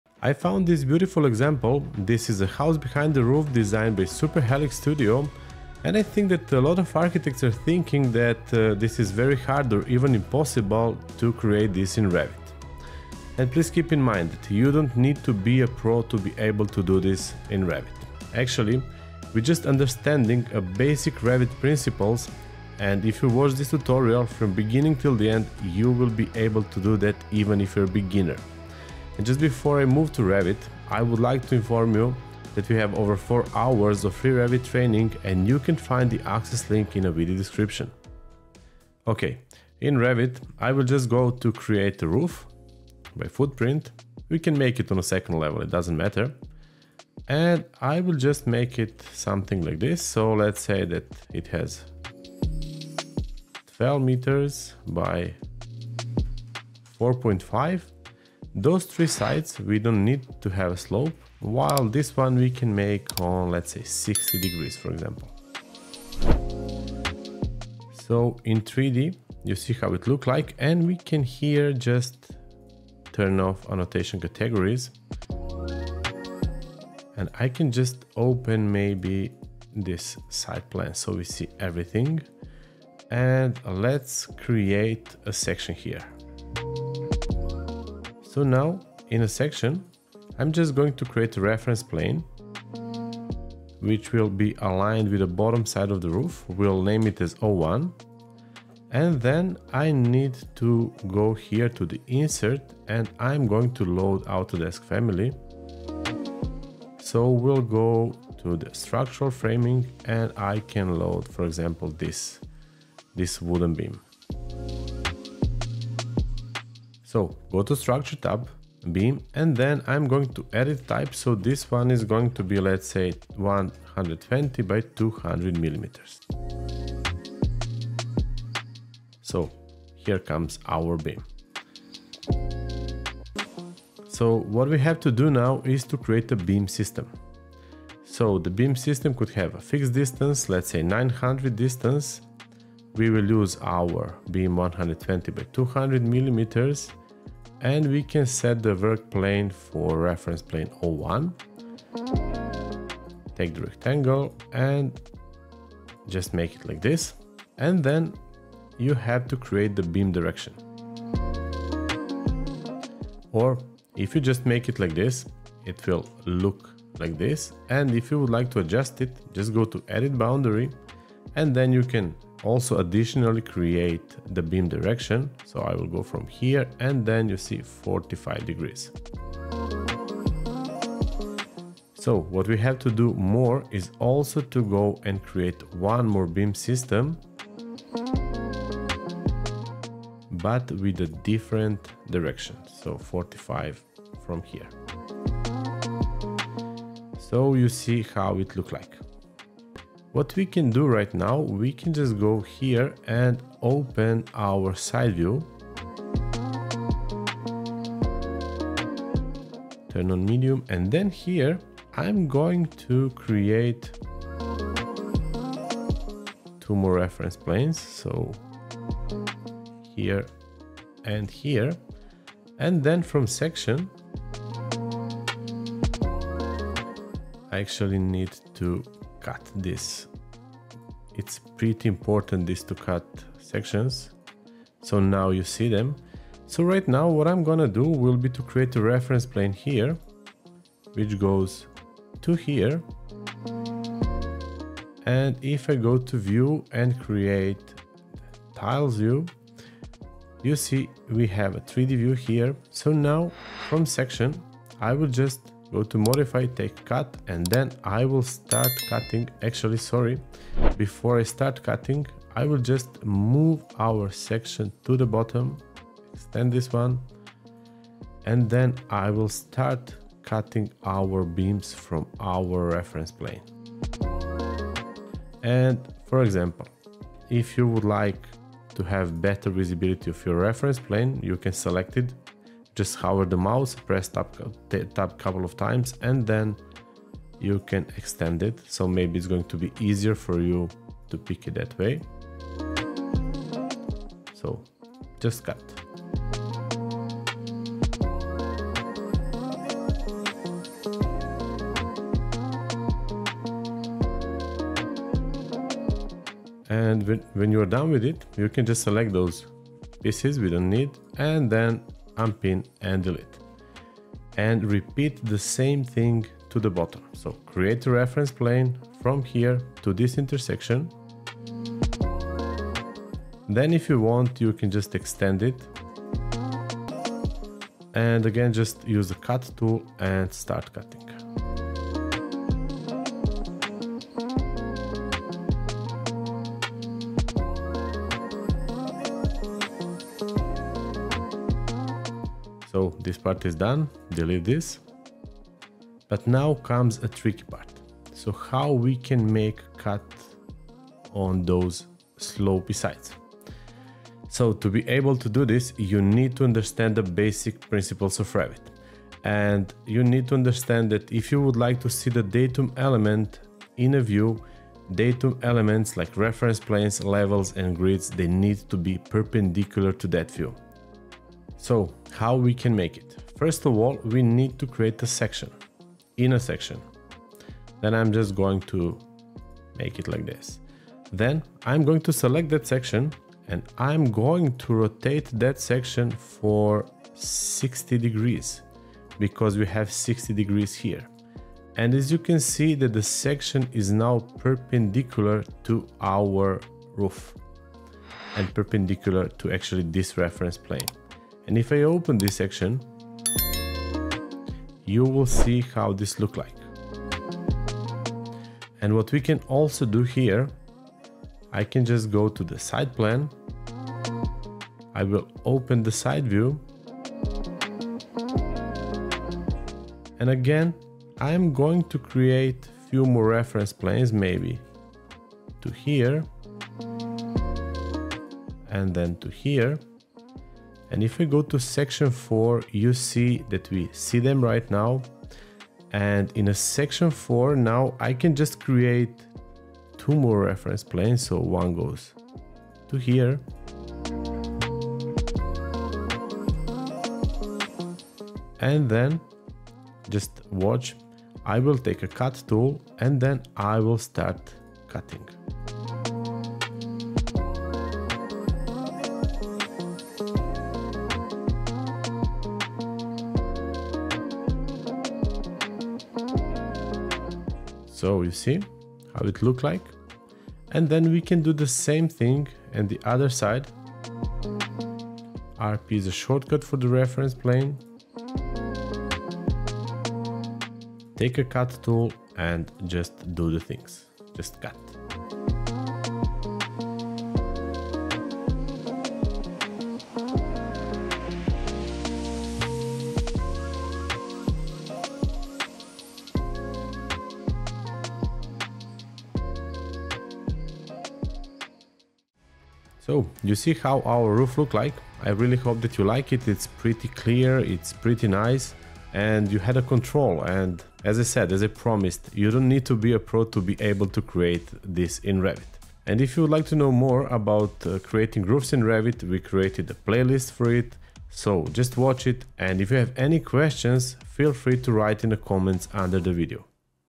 Roof Beams in Revit for Architects
Welcome back architects and architecture students! In this tutorial, you're going to learn the best way of how to create roof beams with slanted edges in Revit. Keep in mind that you don't need to be a pro to be able to create it in Revit. Even if you are beginner and if you follow this step by step tutorials you will be able to create it. This tutorial is packed with tips and tricks of how to create beam systems in revit, to enhance your modeling skills and bring your Revit workflow on a higher level. Let's get started on transforming your architectural visions into reality! Happy modeling!